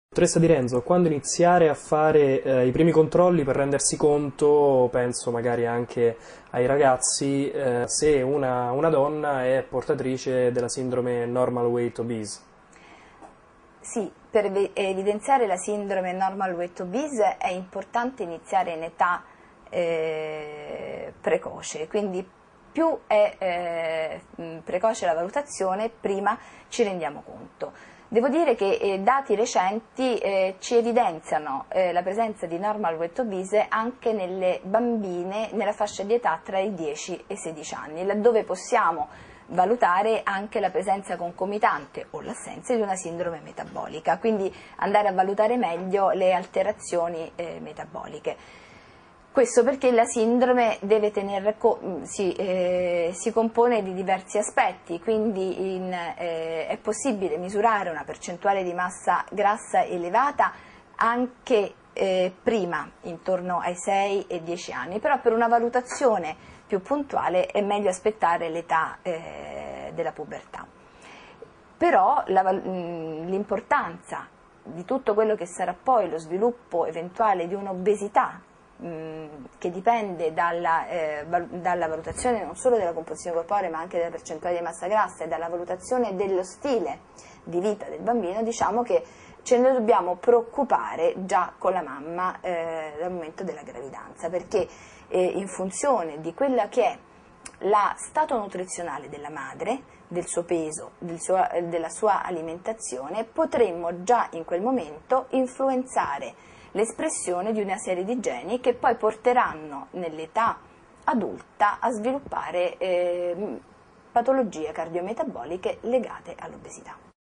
Falsi magri: Sindrome Normal Weight Obese, quando fare i primi controlli
La massa grassa presente anche nei bambini tra i 6 e i 10 anni